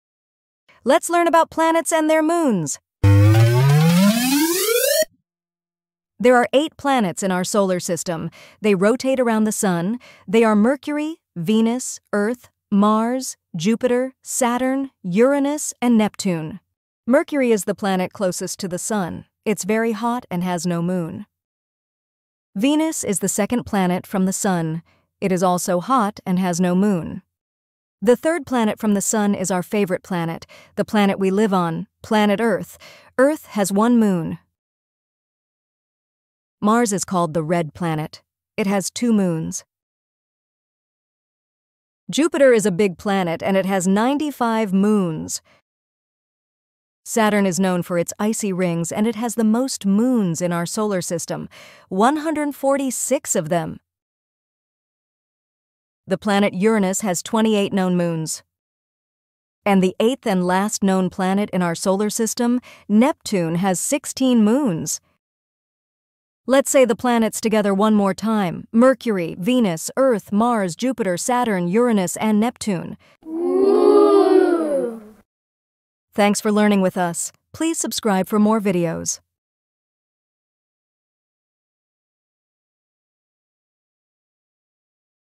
PLANETS AND MOONS! | Kids' Educational Video
Discover the wonders of our solar system with this fun and educational kids' video about the planets and their moons! Perfect for young space explorers, this simple and engaging video teaches children about each planet and how many moons they have.  From the mighty Jupiter with 95 to our very own Earth with just one, kids will love learning these amazing space facts. Join us on this exciting journey through space and spark your child's curiosity about the universe!

**Moon information was taken from the 2024 NASA website.**